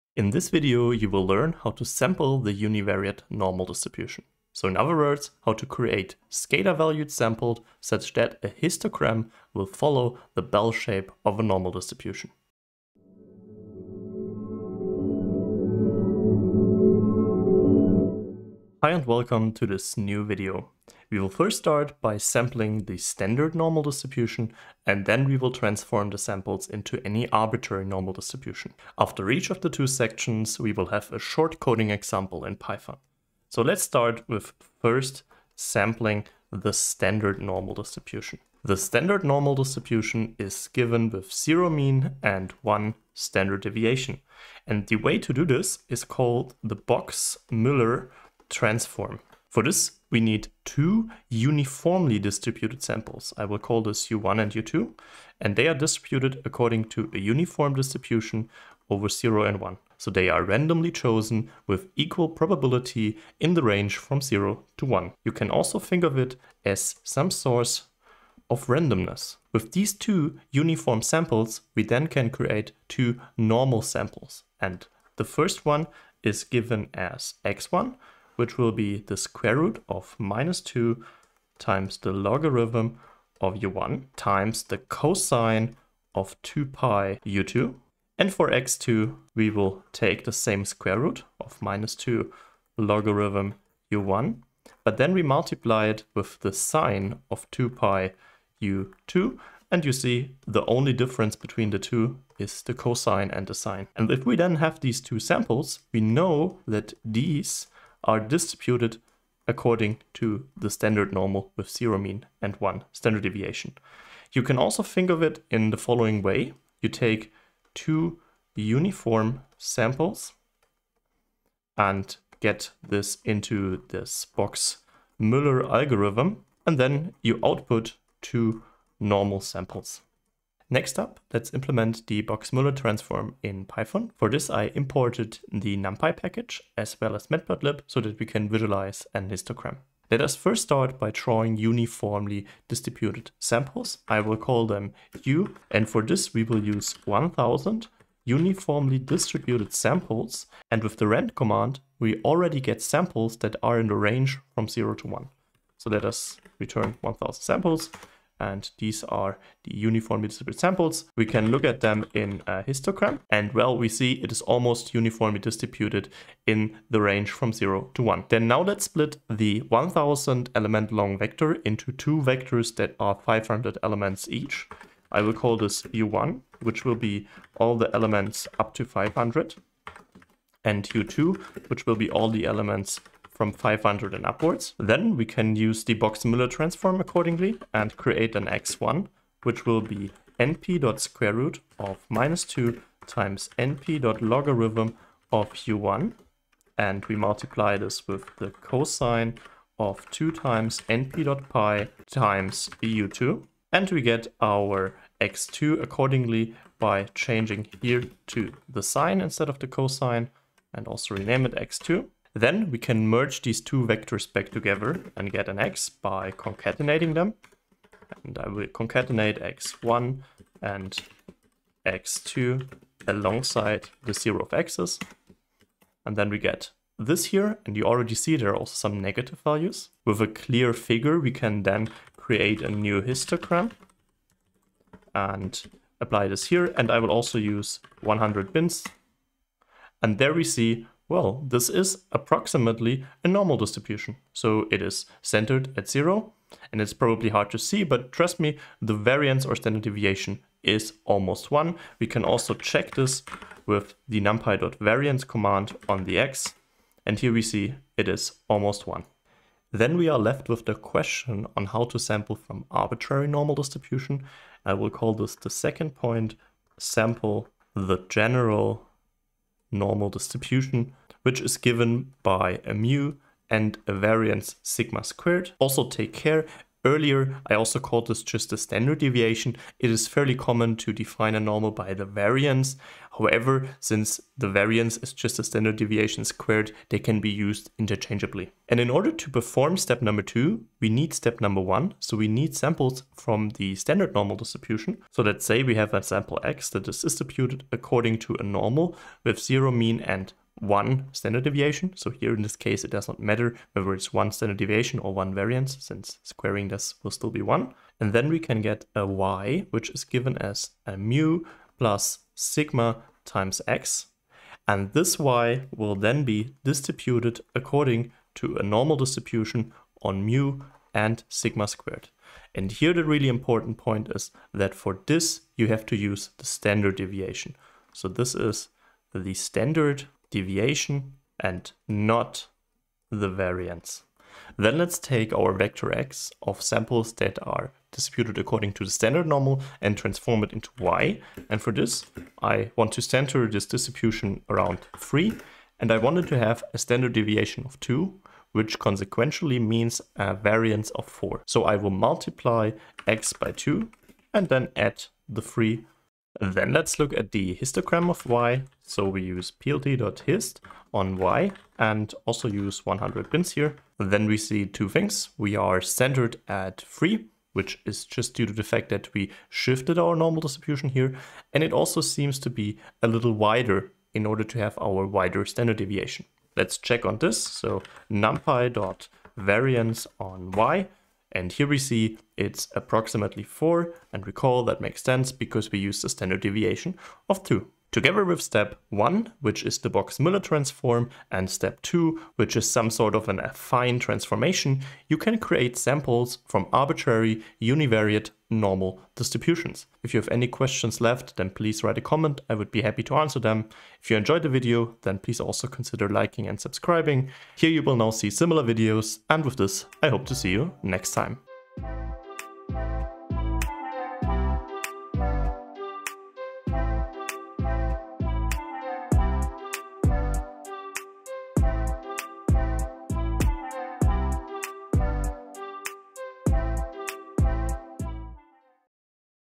Sampling the univariate Normal by Box-Müller Transform | Example in Python
If you have a univariate Normal/Gaussian defined by its mean/mu and variance/standard deviation, how can you draw samples from it? Here are the notes

Have you ever wondered how packages like SciPy or TensorFlow Probability create samples from a (univariate) Normal distribution? There are quite some ways to do so, but one common way that is also computationally very efficient (on high-performance vector computers) is called the Box-Müller Transform. It takes two uniformly distributed samples and generates two normally distributed ones from them.

📝 : Check out the GitHub Repository of the channel, where I upload all the handwritten notes and source-code files (contributions are very welcome)

Timestamps:
00:00 Introduction
00:33 Box-Müller Transform
02:10 Python: Standard Normal
04:19 Transform to arbitrary Normal
05:34 Python: Transformation
06:27 Summary
06:39 Outro